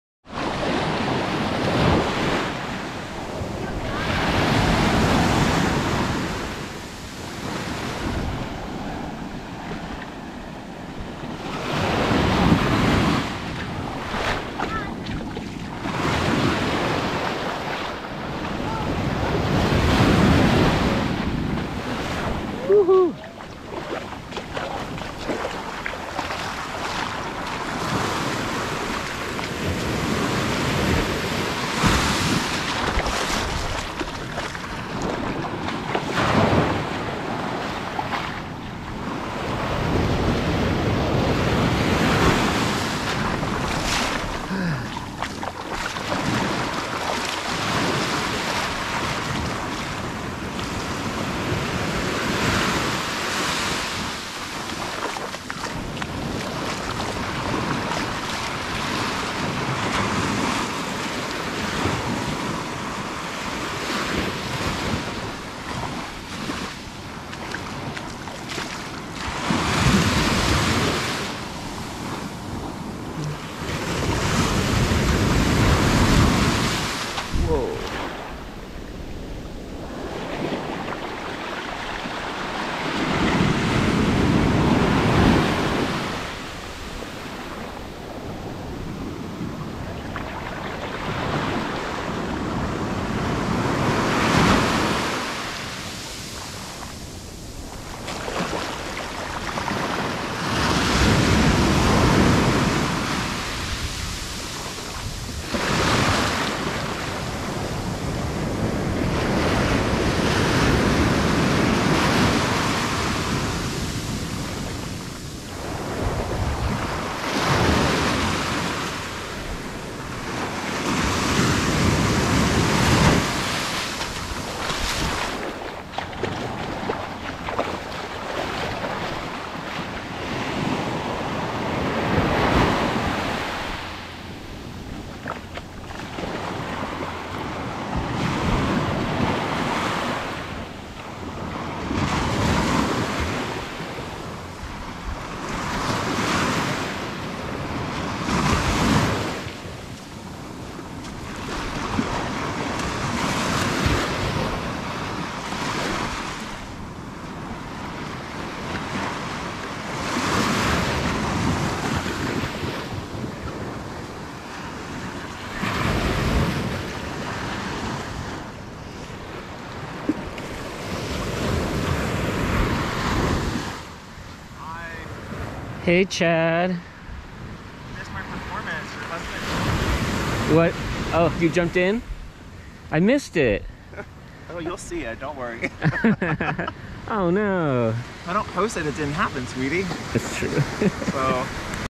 Rehoboth Beach waves and sand (and Chad!) Tuesday, June 21, 2022
Walking along the sand and waves on the Atlantic Ocean at Poodle Beach in Rehoboth Beach, Delaware on Tuesday, June 21, 2022. GoPro Hero9 Black.